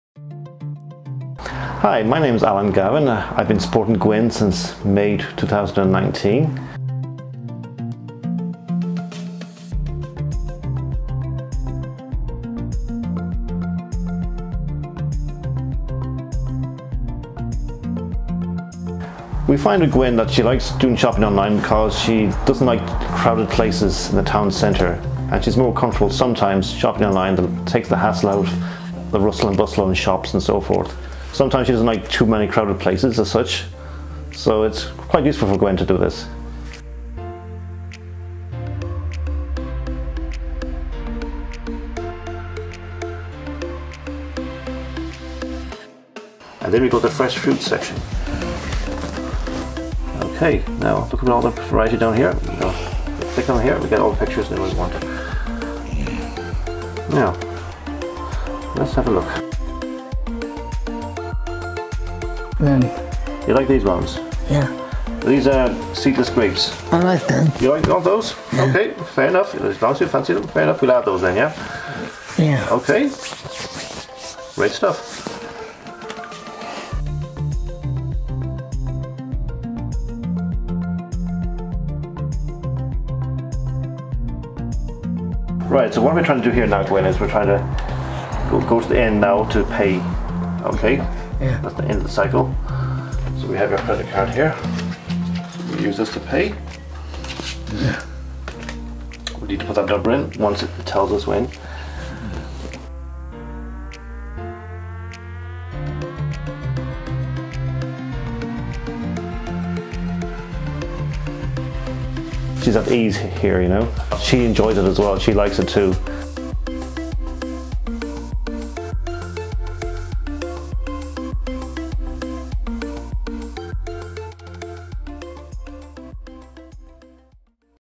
Gwen from Cardiff shops online
We met Gwen from Cardiff who was using technology to do her grocery shop online with the support of Alan.

Digital skills have improved Gwen's life, and enabled her to spend more time doing the things she loves. Watch Gwen's video to find out more about the impact it's had on her.